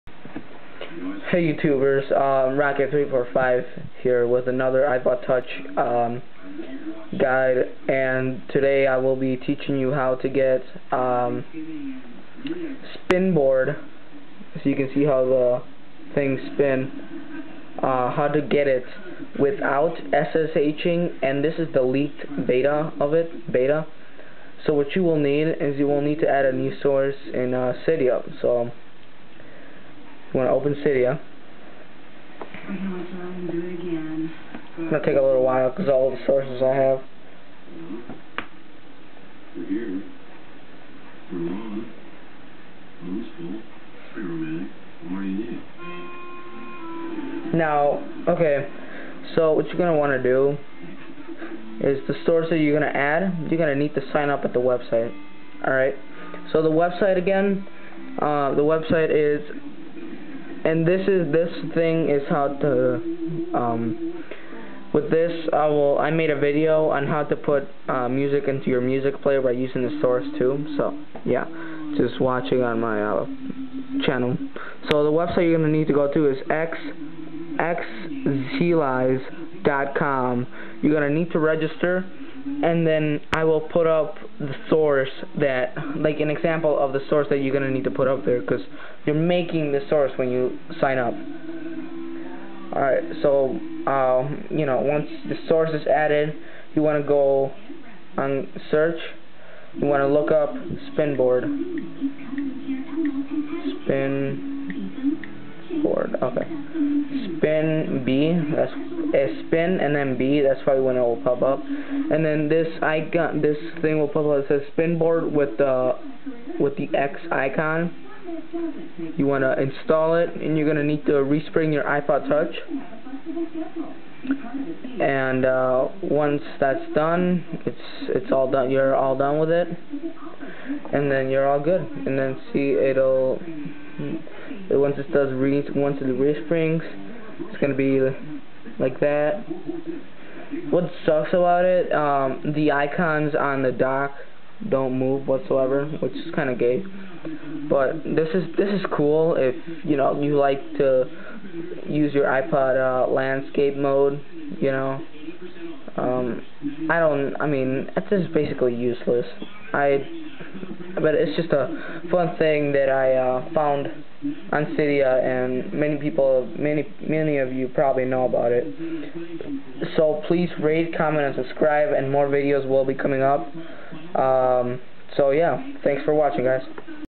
how to get spinboard without ssh on ipod touch, iphone 3g, 2g, 1g
cydia source